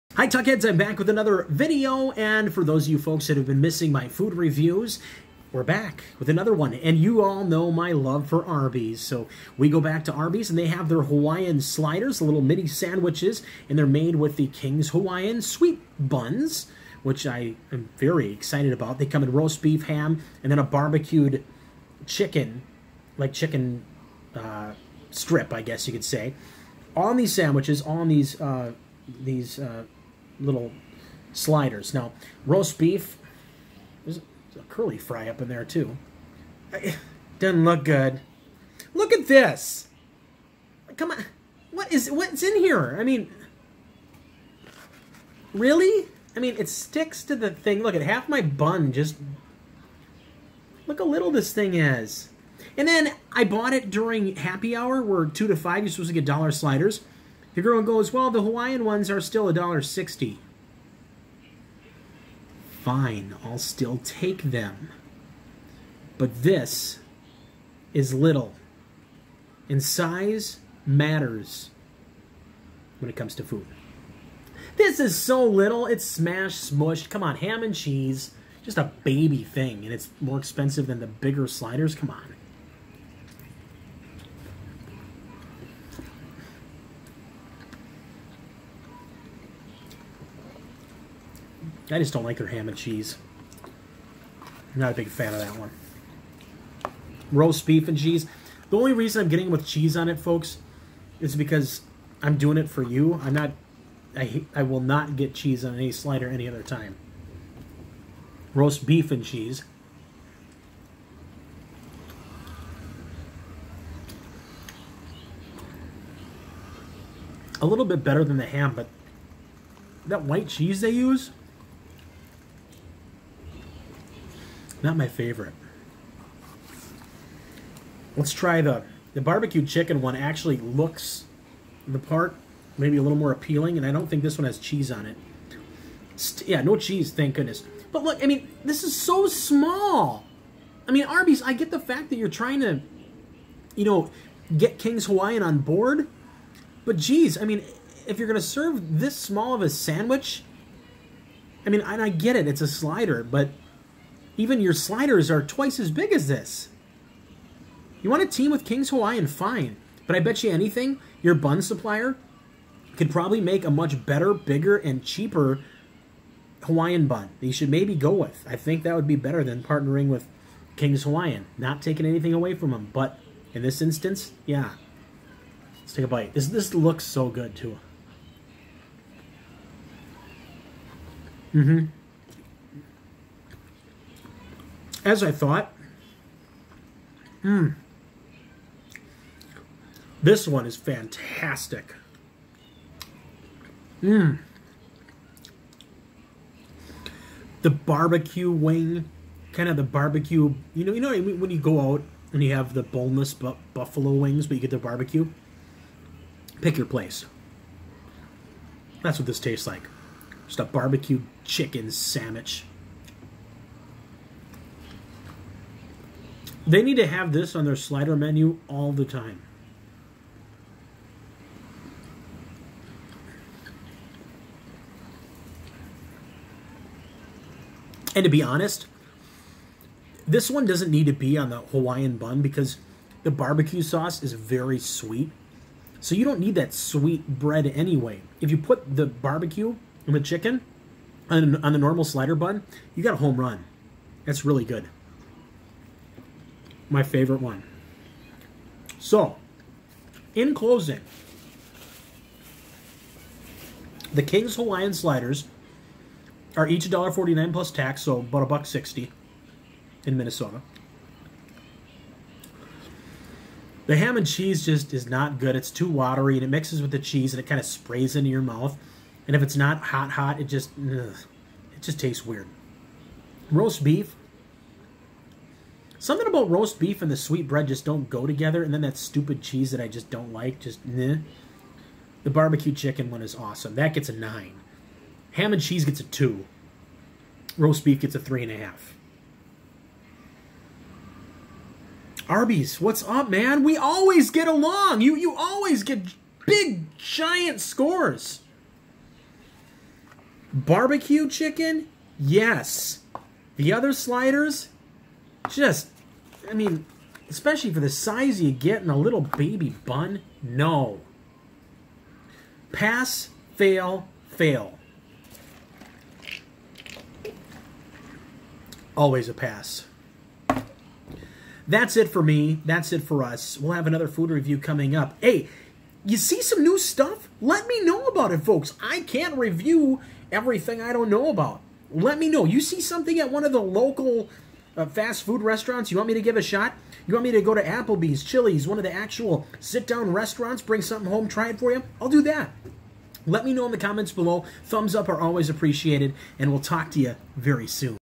Food Review: Tuck Tries All 3 New Arby's Kings Hawaiian Sliders!
Been wanting to do this review for a while now, and finally, got our hands on the Arby's King's Hawaiian Sliders! (Roast Beef, Ham, BBQ Chix) Was it worth the wait??